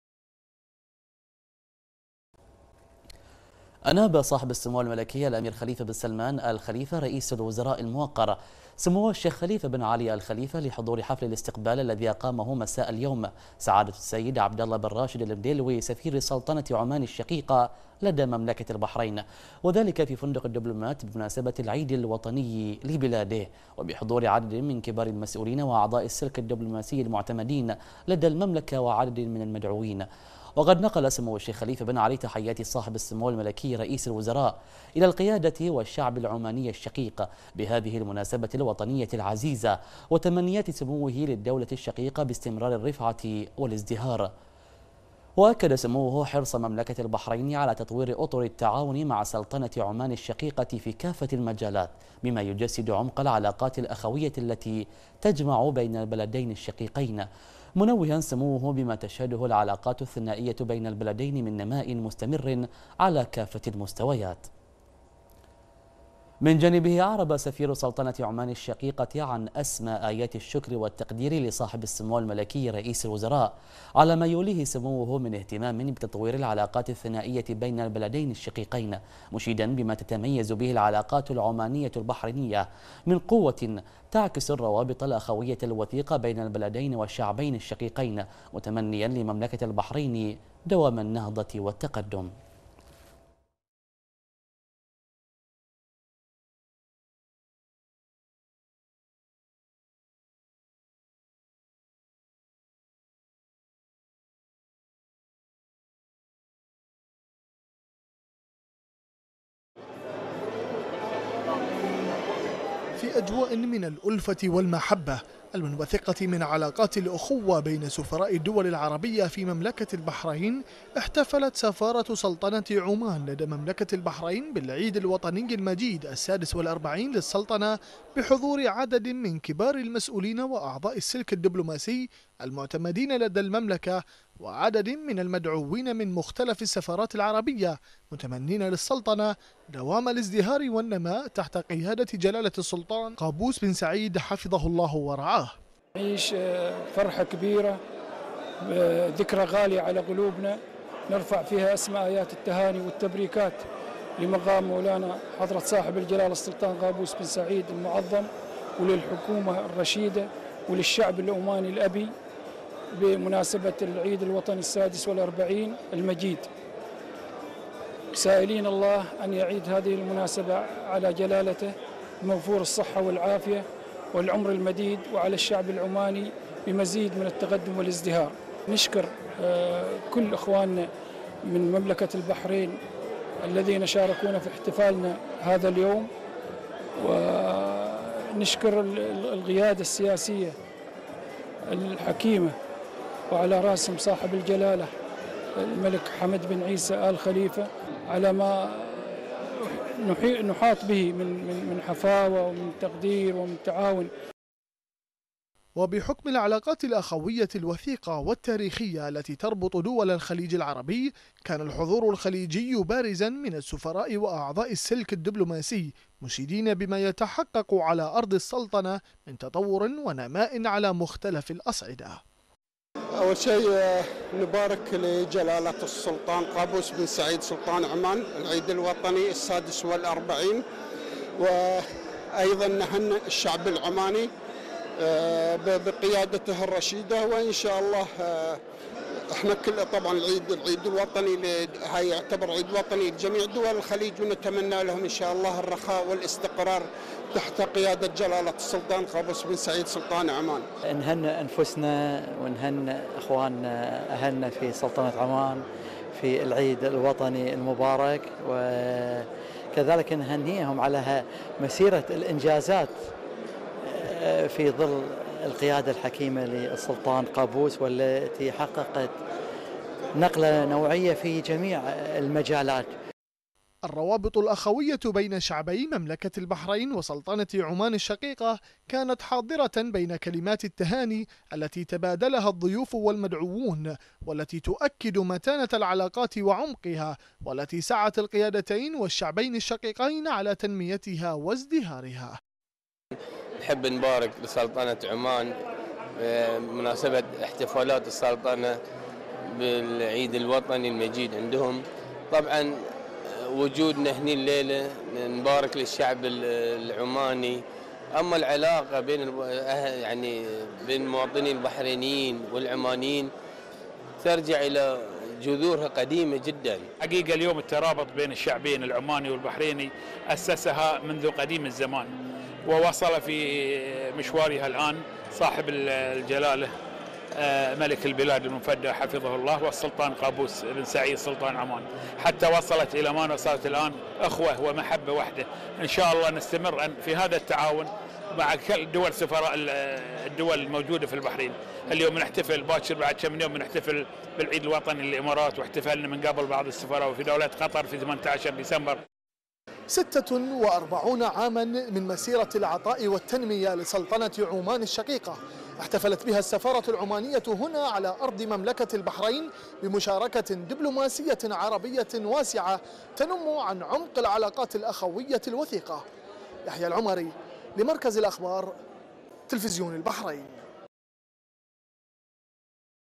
البحرين : نيابة عن سمو رئيس الوزراء..سمو الشيخ خليفة بن علي يحضر حفل السفارة العمانية
نيابة عن سمو رئيس الوزراء..سمو الشيخ خليفة بن علي يحضر حفل السفارة العمانية